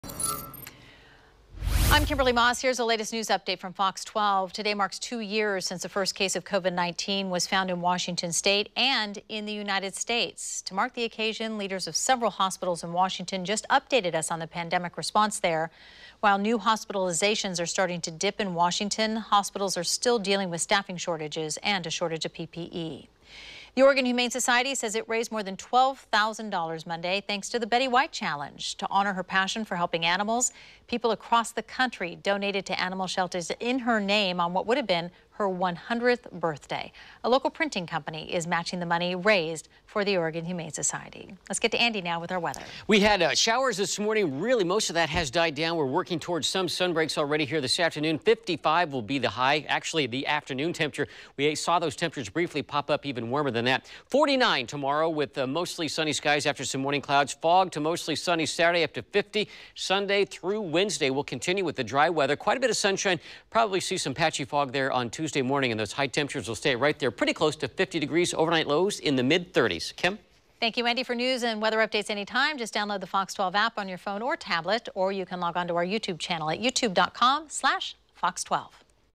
News Update: Two years since first COVID case reported in Washington, OHS raises more than $12K
Here are the latest headlines and weather forecast from FOX 12 for the afternoon of Thursday Jan. 20, 2022.